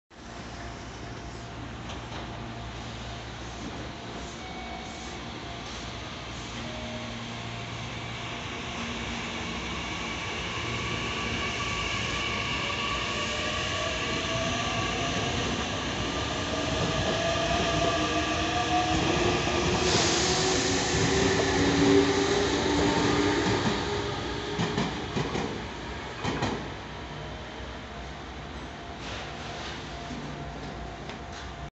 名鉄9500系9505F＋3100系3107F 急行豊橋行 知立駅発車
2021年3月5日（金）撮影
名鉄名古屋本線知立駅にて